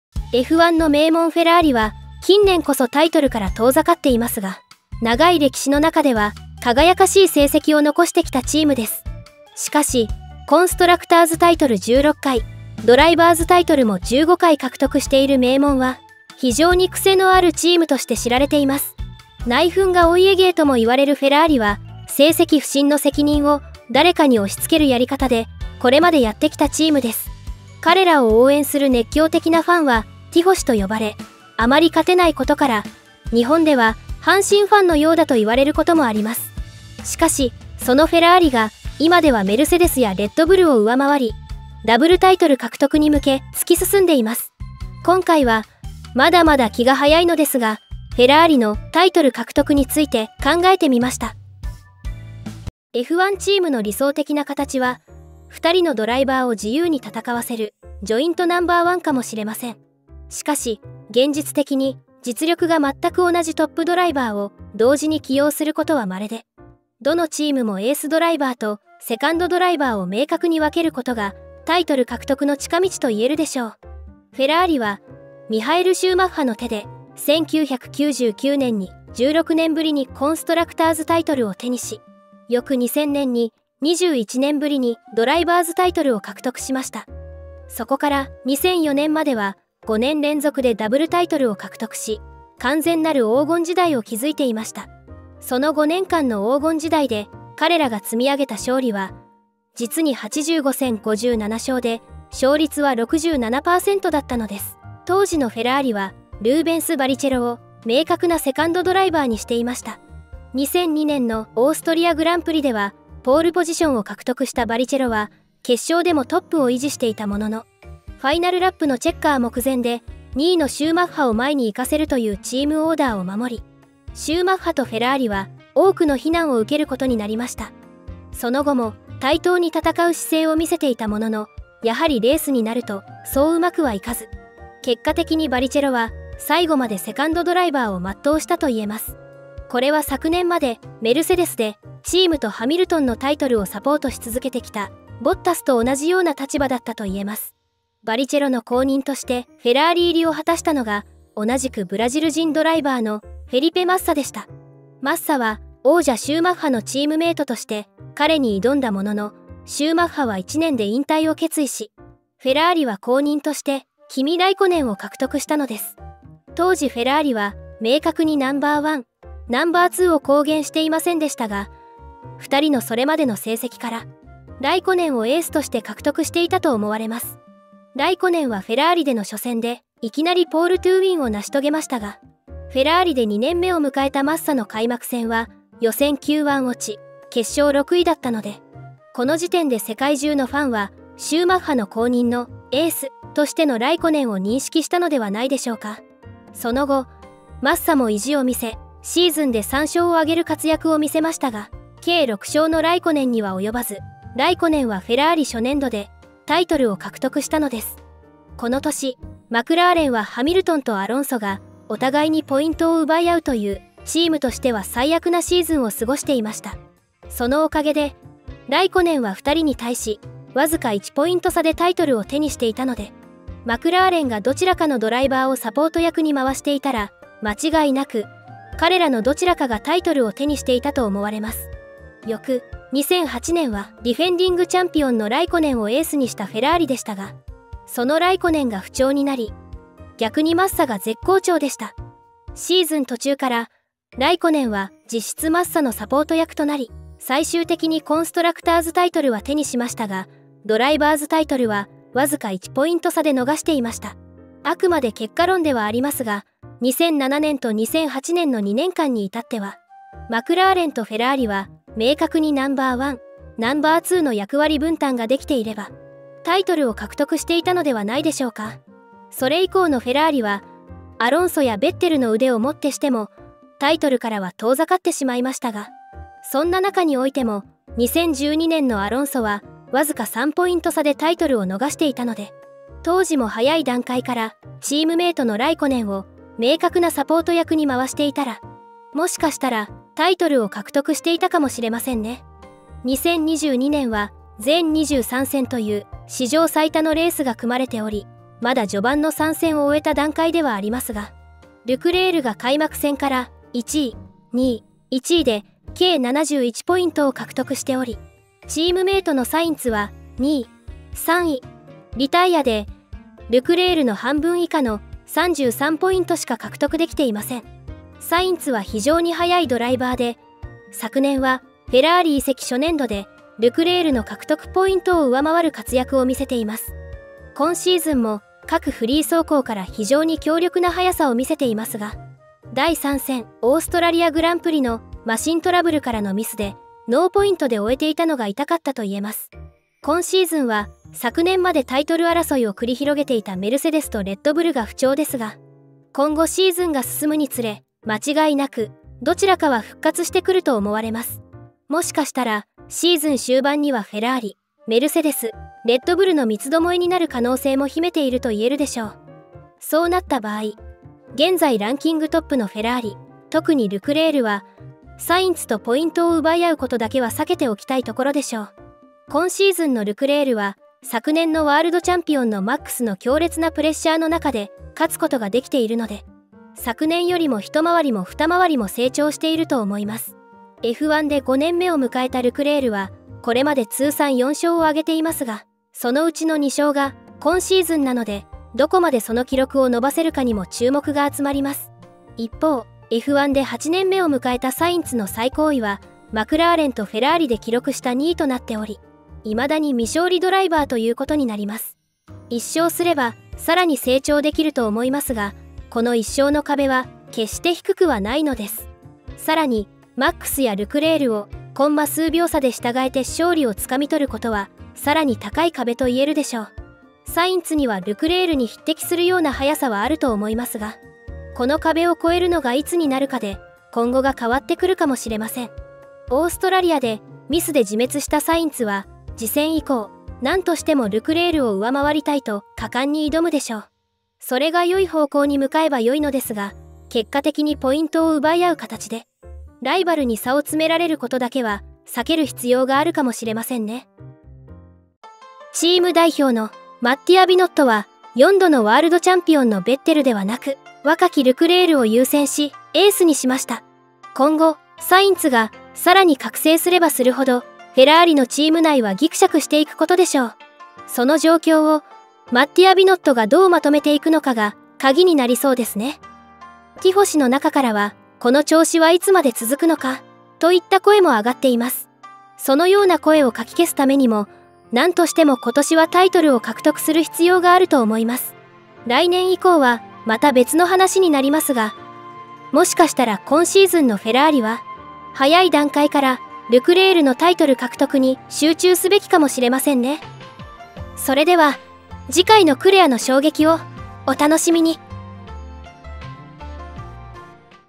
【F1 2022】フェラーリは冷酷なチームオーダーでルクレールを最優先できるのか？
F1の名門フェラーリは、近年こそタイトルから遠ざかっていますが、
長い歴史の中では、輝かしい成績を残してきたチームです。
しかし、コンストラクターズタイトル16回、ドライバーズタイトルも15回獲得している名門は
非常にクセのあるチームとして知られています。
今回は、まだまだ気が早いのですが、フェラーリのタイトル獲得について考えてみました。

●DAZNのご案内
　　F1、F2を楽しみたい方はこちらから登録↓

目次
00:00 オープニング
00:51 本編
07:24 エンディング

BGM / 若林タカツグ/カッパエンタテインメント